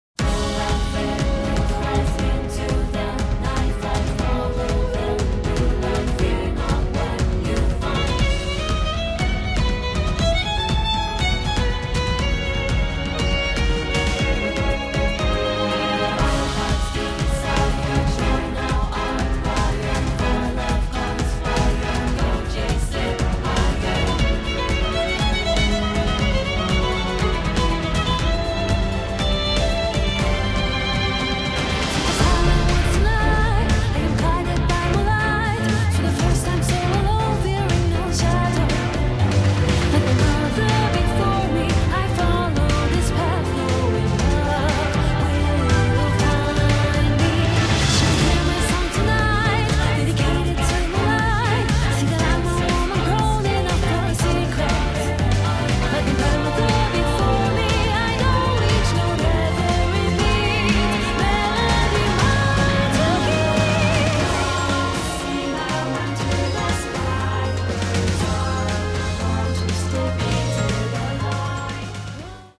90's Magical Girl Room - Apartment
This dream in pink and purple will allow you to live out your childhood dream of being a Magical Girl. Sailor Moon Costume not included.